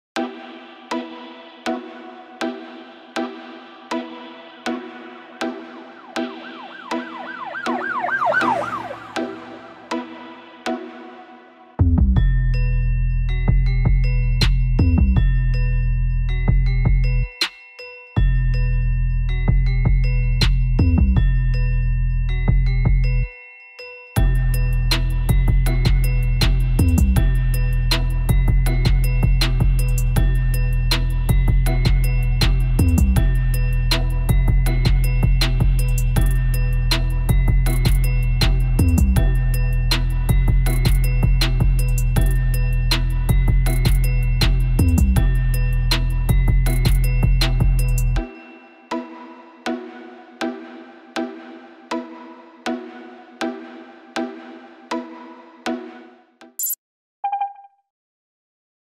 Raperos mantienen la tradición ambulante
El “genere” como le llaman estos raperos al sustento que consiguen de sus rimas, es la base económica de muchos hogares.